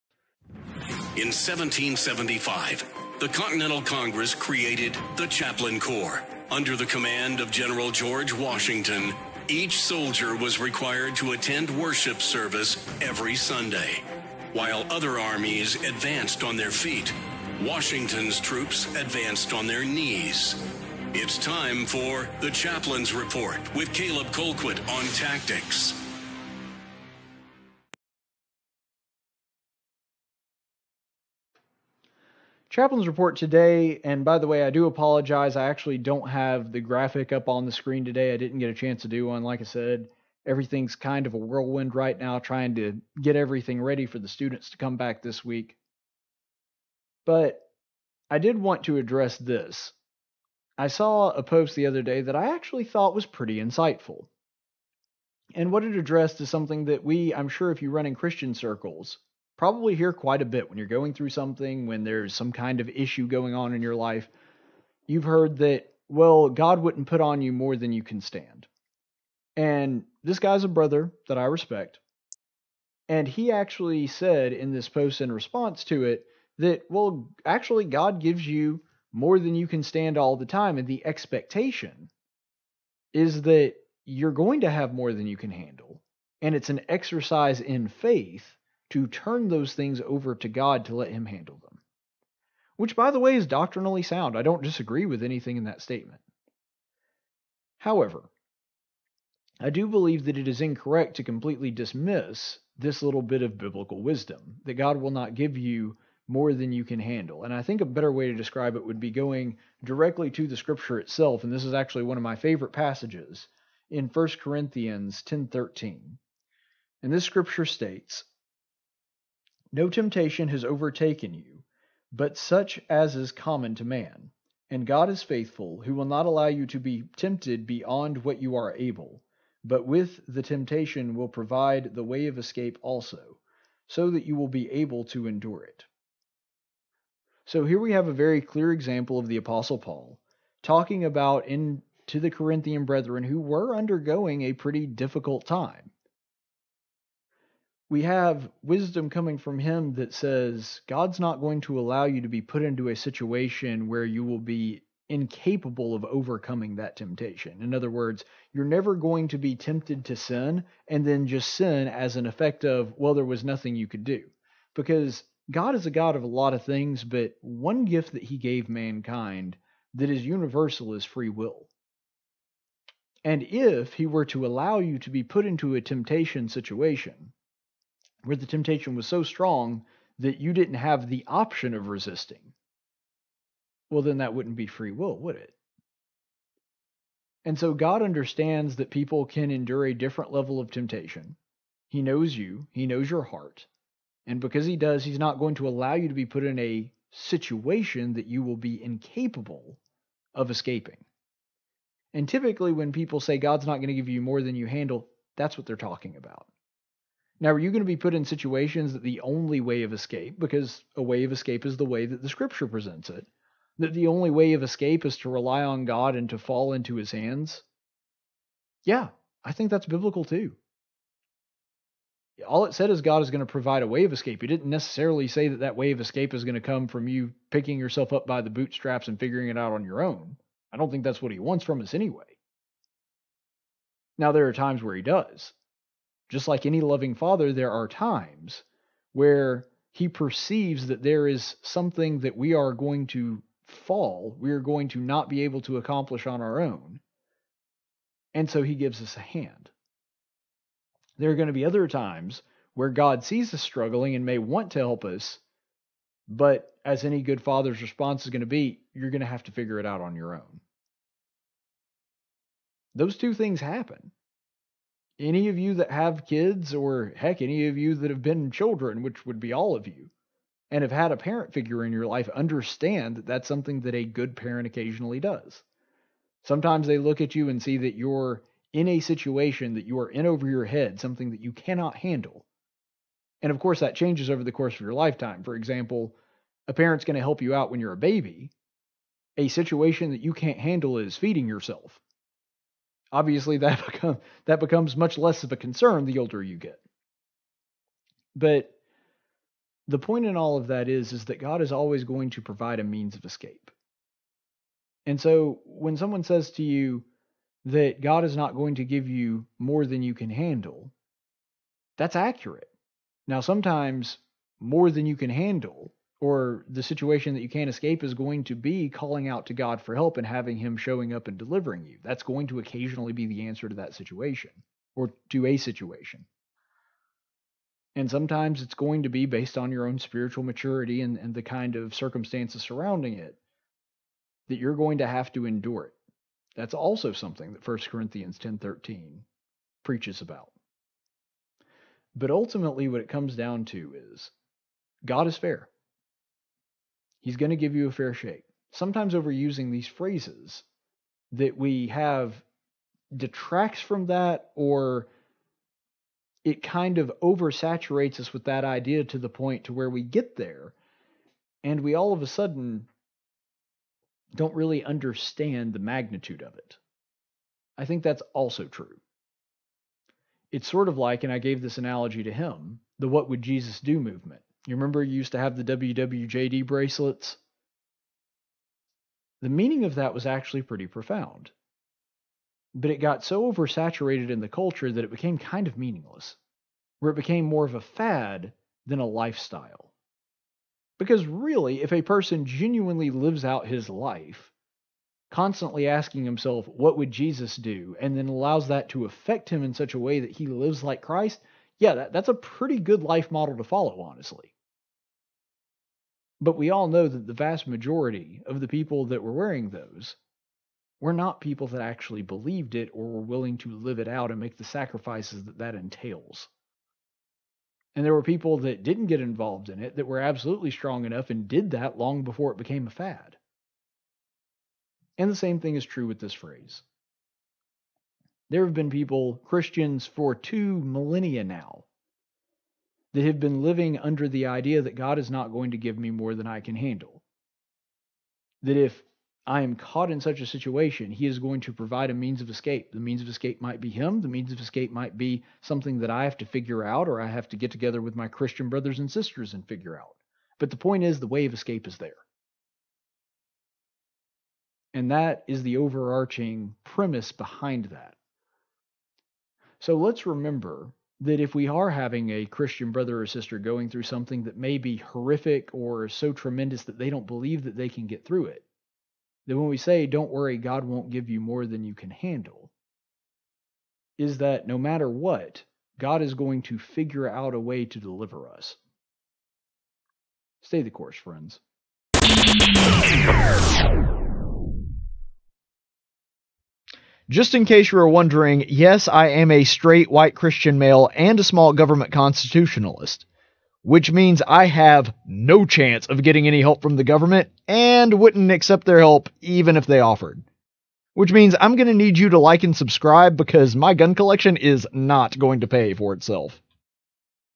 Chaplain's Report- More Than You Can Handle
When asked about the cliche "God won't give you more than you can handle," Caleb explores I Corinthians 10:13 to see if it is true.